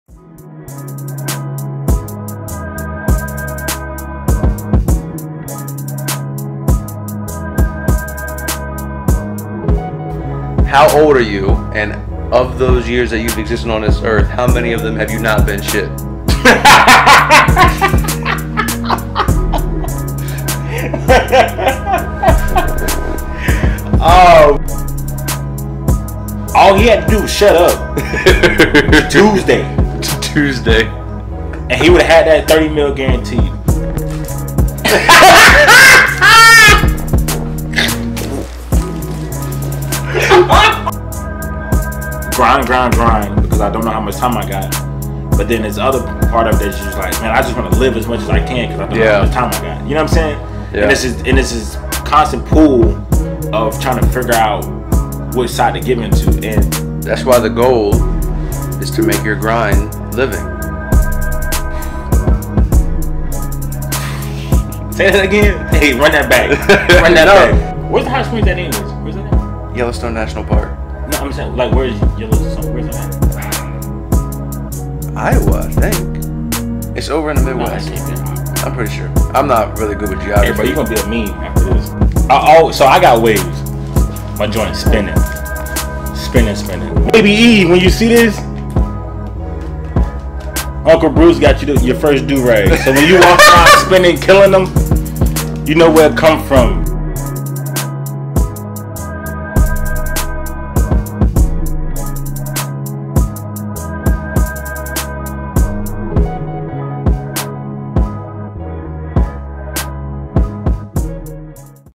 Episode 1 Recap
A Quick Recap from Episode 1 of TATU- Tools And Their Uses